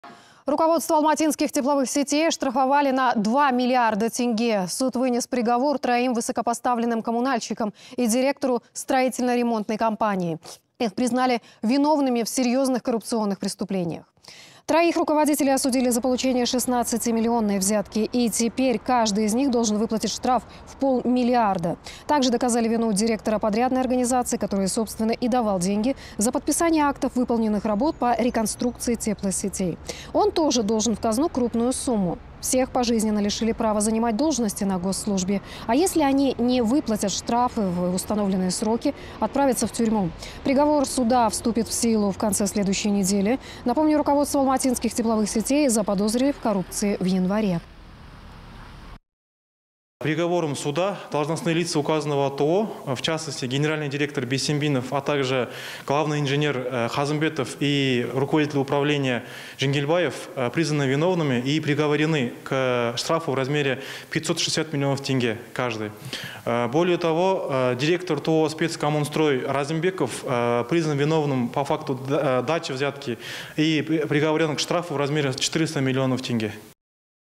К штрафам в полмиллиарда тенге приговорили коммунальщиков в Алматы
Руководство «Алматинских тепловых сетей» оштрафовали на 2 миллиарда тенге. Суд вынес приговор троим высокопоставленным коммунальщикам и директору строительно-ремонтной компании. Их признали виновными в серьезных коррупционных преступлениях.